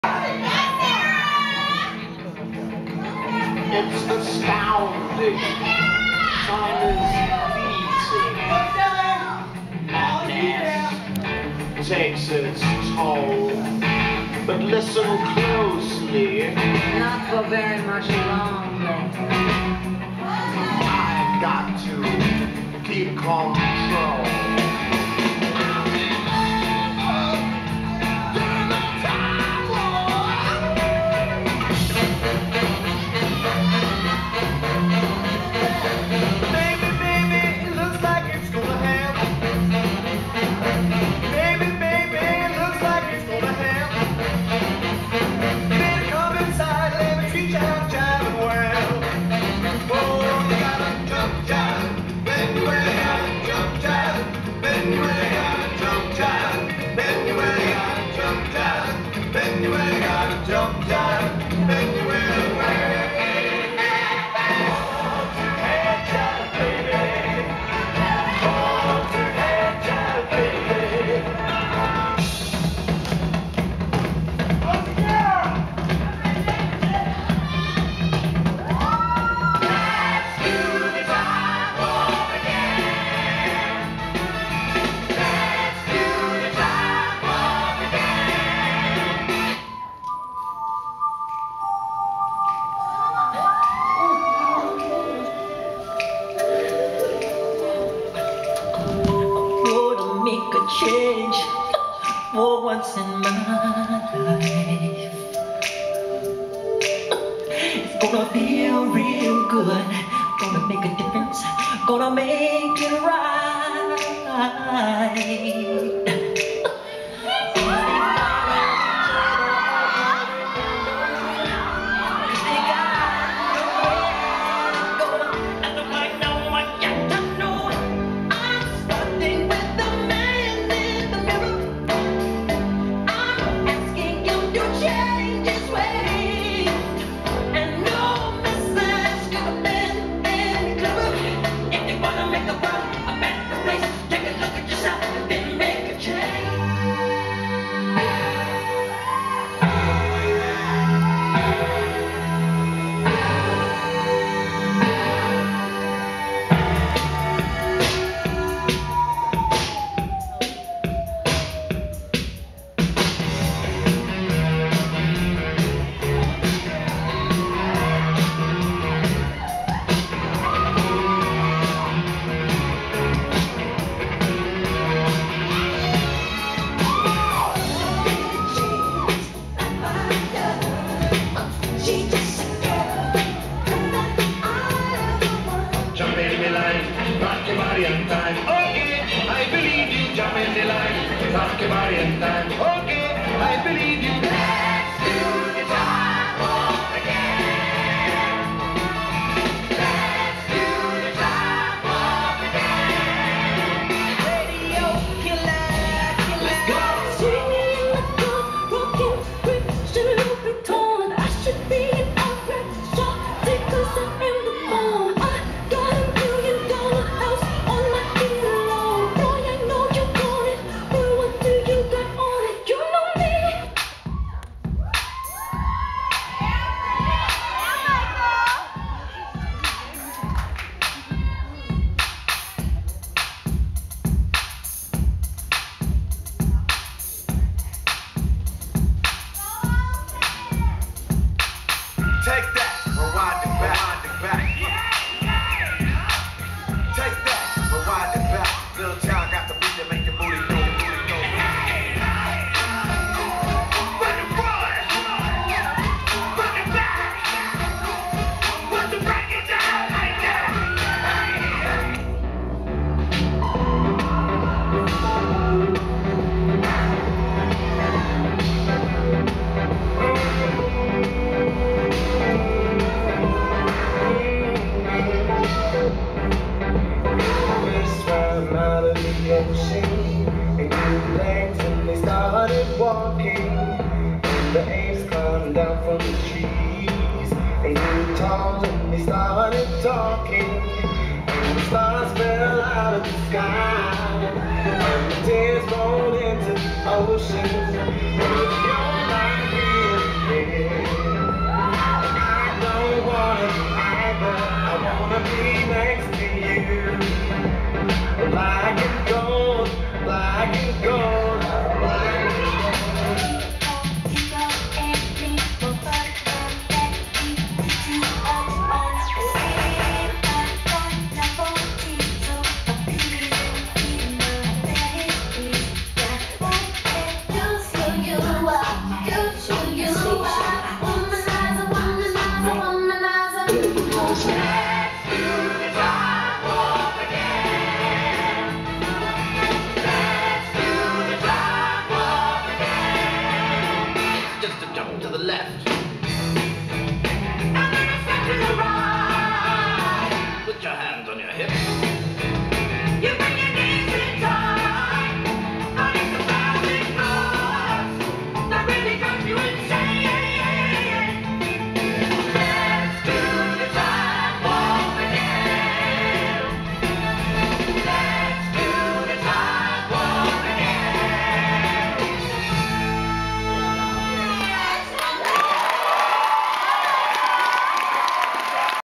Strike a Pose - Intermediate Dance Routine
Irvine High School's "Strike a Pose" dance show.  Sara dancing with her intermediate dance class.